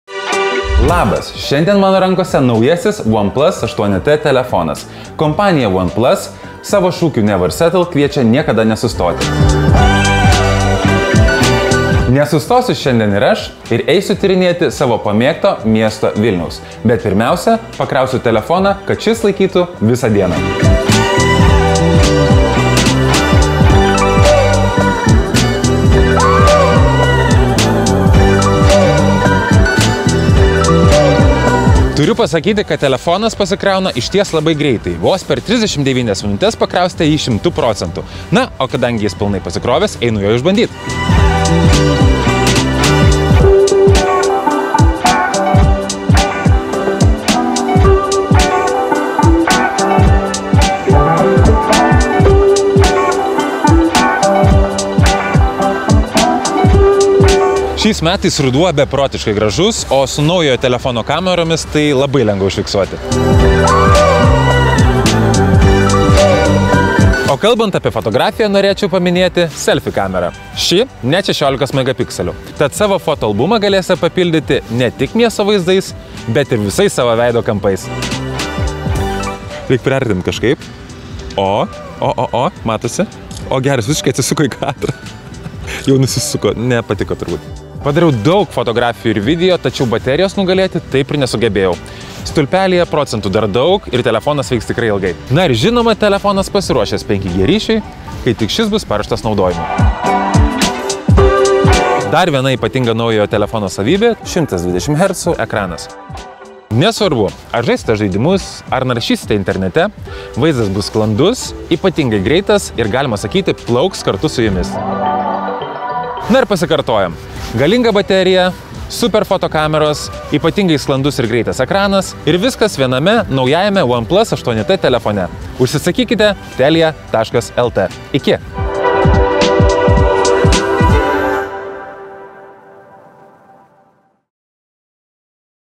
Telia ekskursija po rudeninį Vilnių – su „OnePlus 8T“
Dar spėjome pagauti paskutines rudens akimirkas ir jas užfiksuoti net su penkiomis „OnePlus 8T“ kameromis, tarp kurių: net 46 MP plataus kampo ir 16 MP priekinė. O kur dar 120 Hz ekranas, kuriame bet kas atrodys kaip kino teatre. „Never Settle“, sako „OnePlus“ gamintojai – ir tikrai, šimtai lapų spalvų atrodo lyg tikros.